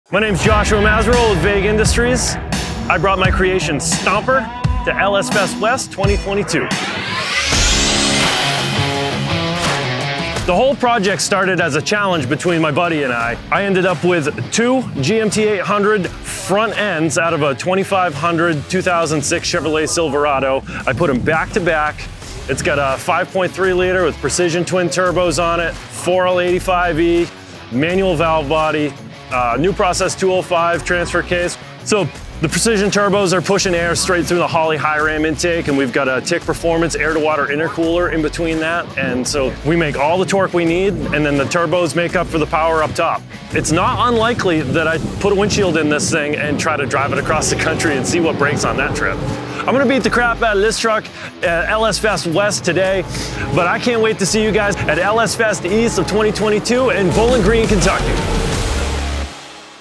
LS Fest West 2022: Vague Industries’ Four-Wheel Spin-Cycle Kei Truck Called “Stomper”
Vague Industries’ Josh Mazerolle is no stranger to wild, creative and off-the-wall builds. His latest creation is a twin-turbo, four-wheel drive, four-wheel steer, 100% insane take on a 1980 Mitsubishi kei truck. He calls it “Stomper”, and it cranks out donuts like you wouldn’t believe.